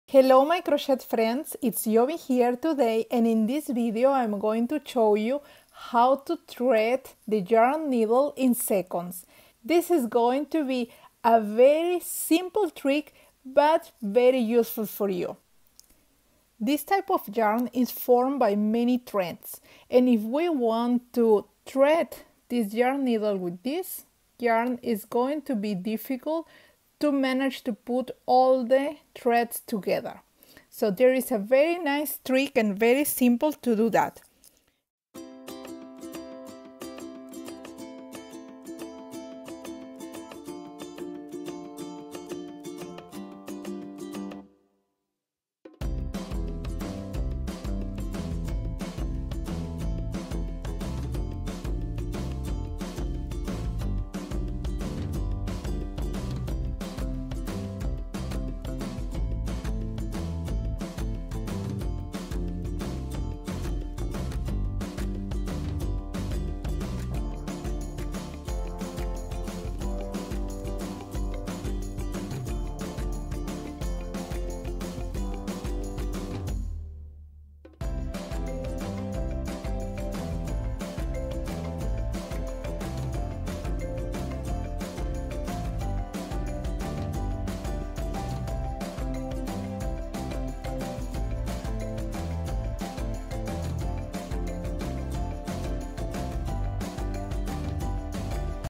The EASIEST WAY to THREAD a YARN NEEDLE
Hello my crochet friends! Today I would like to show you a quick trick how to thread the yarn needle in seconds, this is the easiest way ... takes just few second